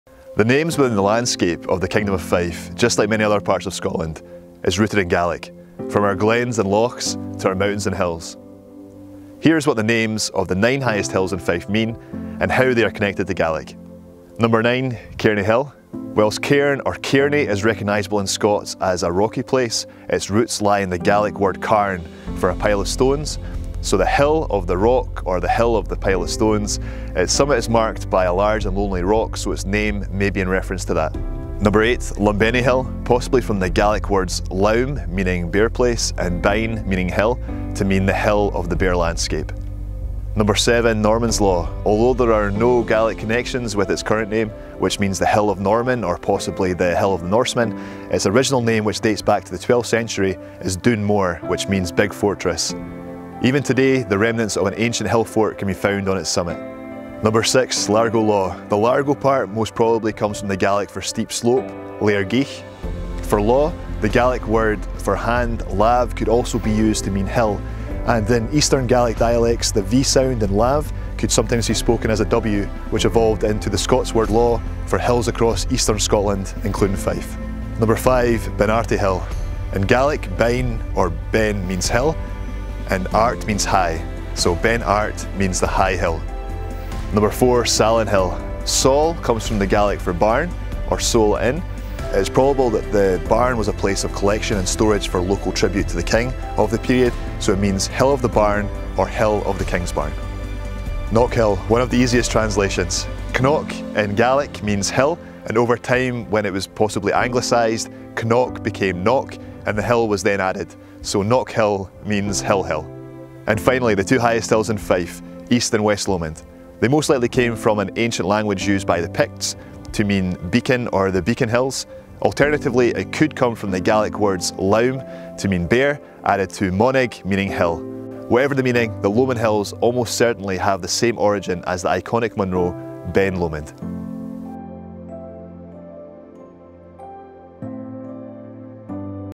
The meaning of Fife hills names and their Gaelic connection
Thanks to help from Fife Council's Gaelic in Fife, I can reveal what the Kingdom's nine highest hill names mean and how are all connected to the Gaelic language.